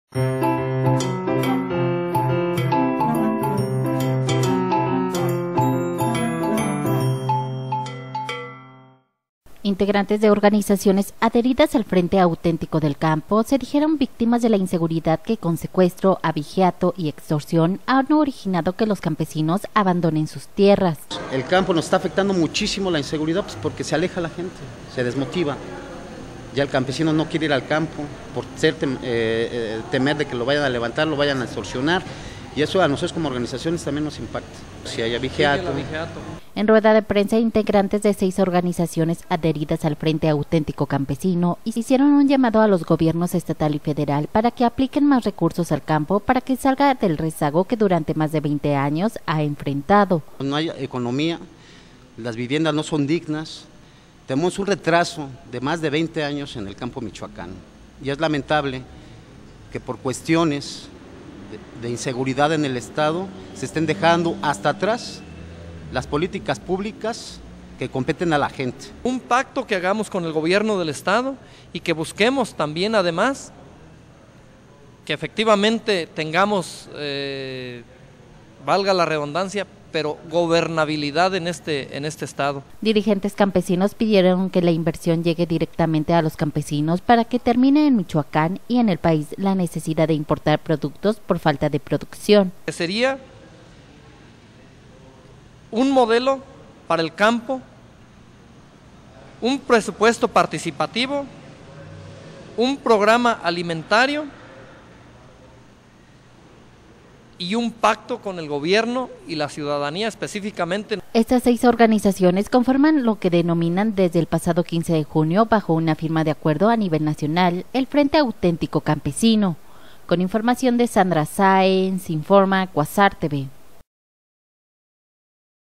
Alto a la inseguridad en Michoacán, piden dirigentes campesinos
Integrantes de organizaciones adheridas al Frente Auténtico del Campo, se dijeron víctimas de la inseguridad que con secuestro, abigeato y extorsión han originado que los campesinos abandonen sus tierras.